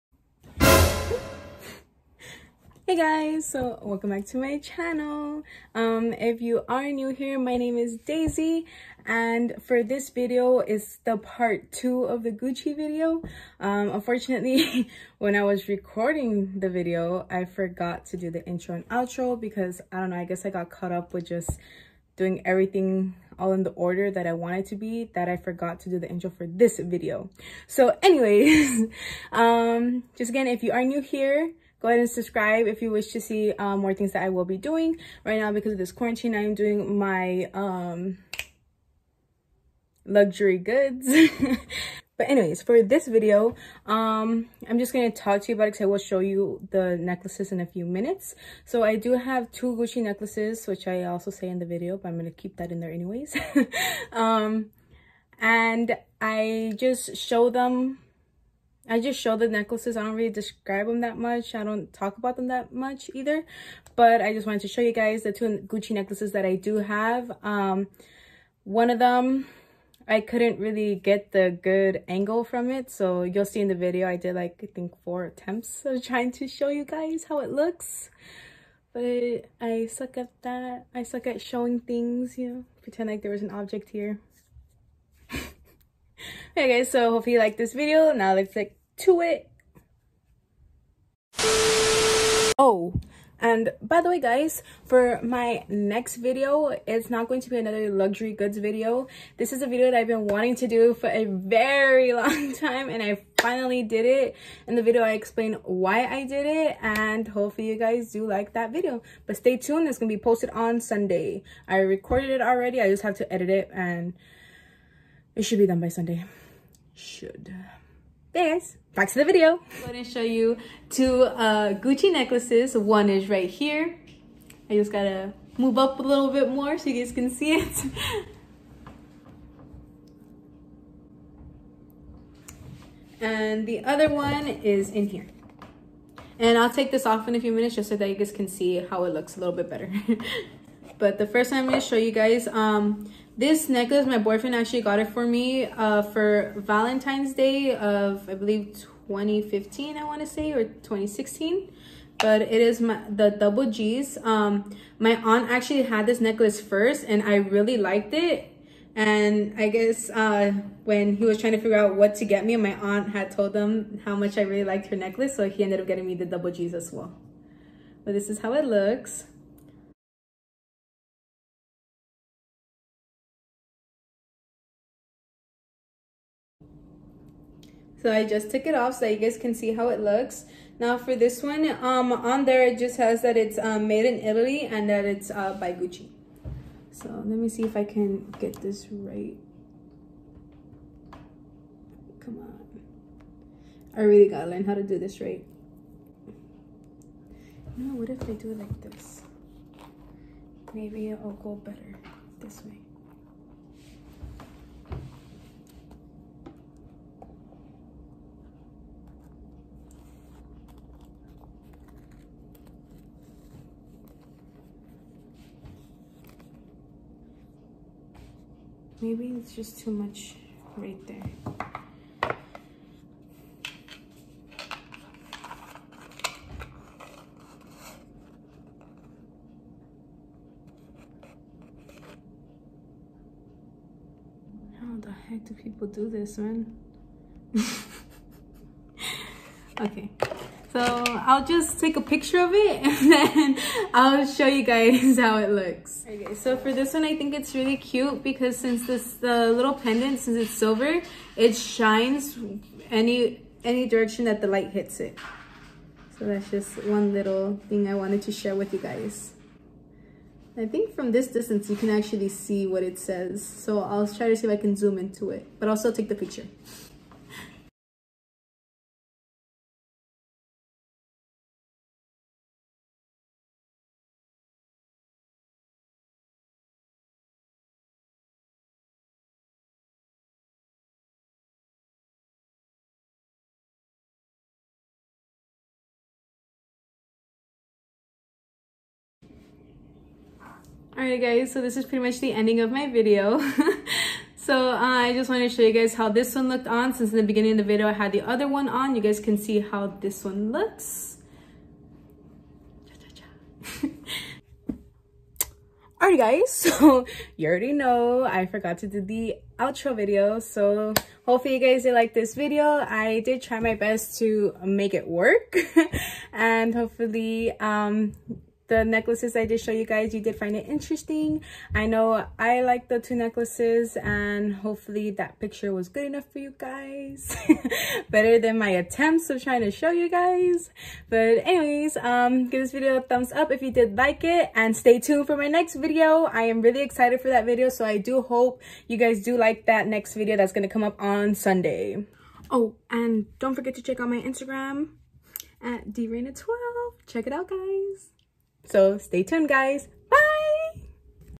GUCCI: Double G Necklace + "made in Italy" necklace in silver| Gucci Jewelry Pt.2
Hello Everyone !!
I do Hope you like this video, I know it seems a little weird because I forgot to do the intro and outro. But I made it work I think, I wasn't so sure on what to say in the video but I just wanted to share with you all my Gucci collection. I know it is not much but it's a start !